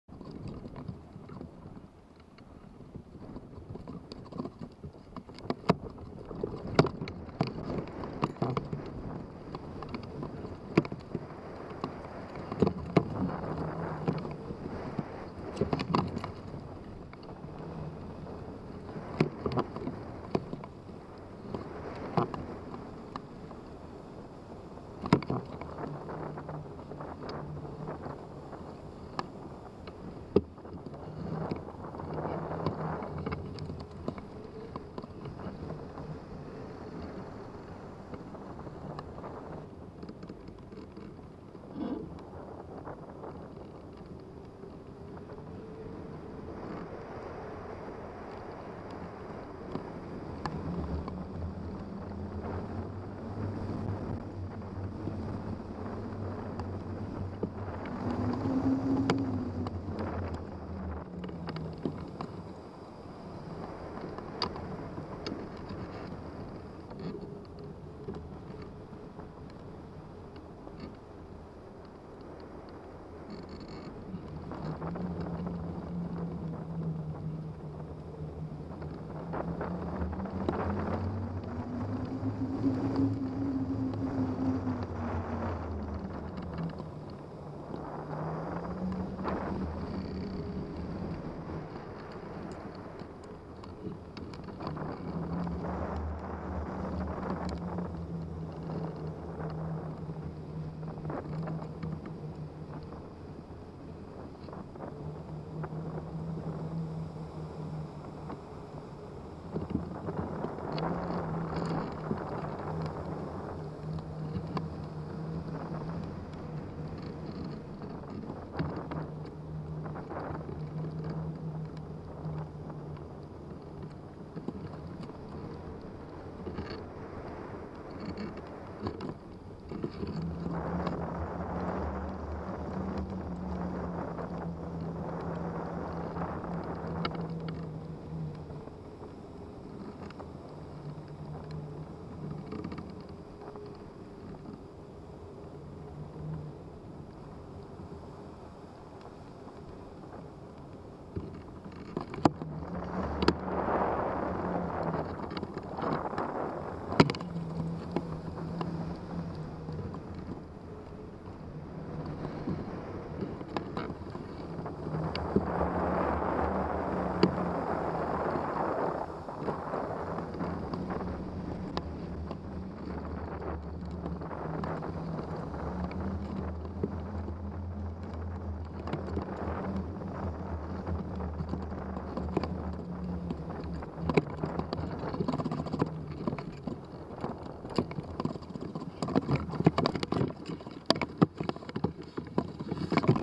Hang gliding Seal Rocks NSW NE facing side on my Discovery 195
This video is about hang gliding Seal Rocks NE side. Nice place to launch from and you can fly to a much larger ridge to the north. Can also fly in a NNE over the village of Seal RocksNSW. I have also placed some wool over the mic and this has reduced the wind noise considerably.